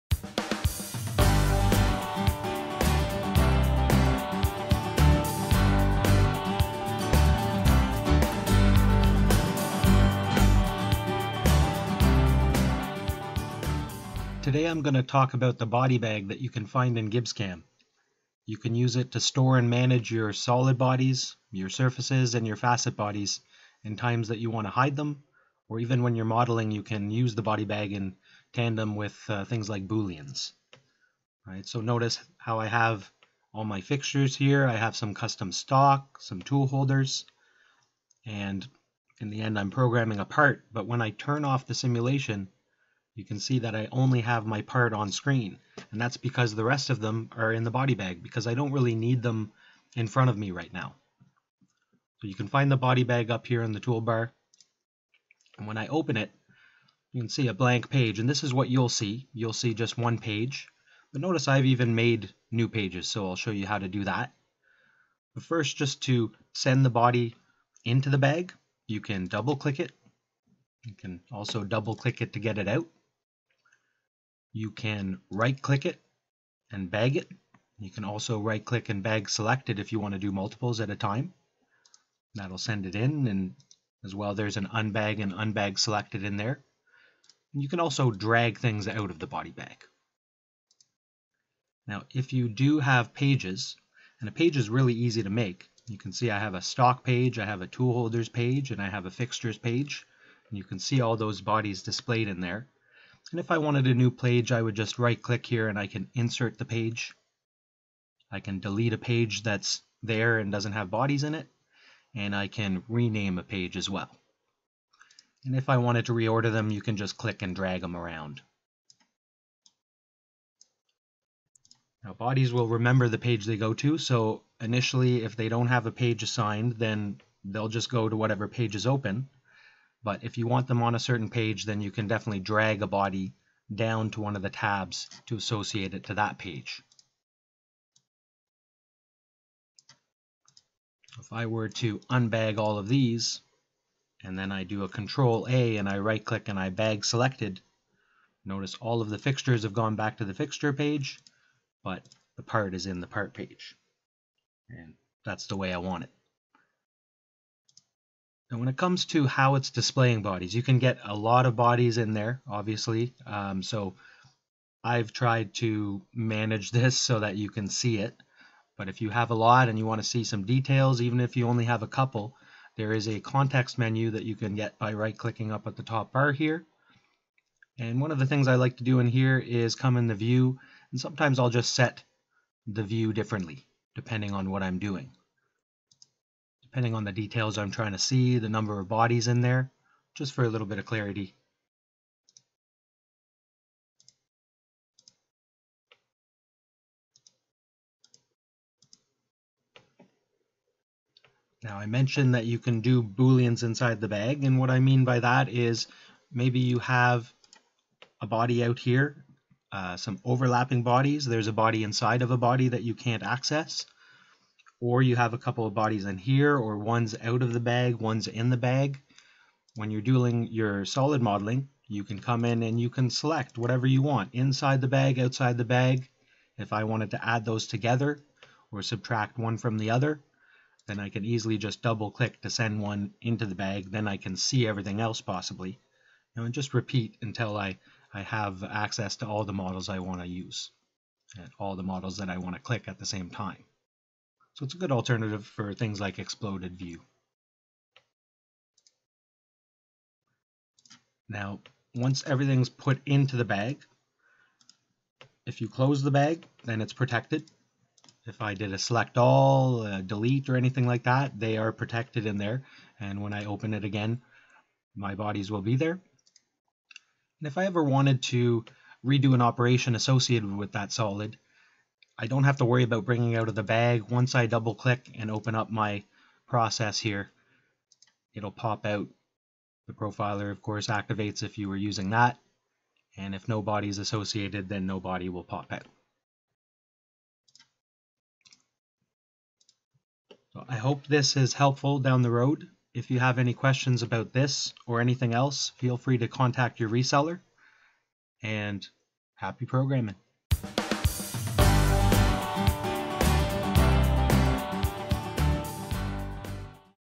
GibbsCAM Tech Tip: How to Use the Body Bag
This month’s GibbsCAM Tech Tip shows you how to use the Body Bag.

Despite its rather humorous name, the Body Bag is very useful for storing, organizing, and managing solid bodies. You can store and organize bodies in any way you find useful, such as by type, with separate pages for stock, part, and fixtures.

The Body Bag keeps the screen uncluttered, making it easy to access the features you want to create toolpath for, without other bodies getting in your way.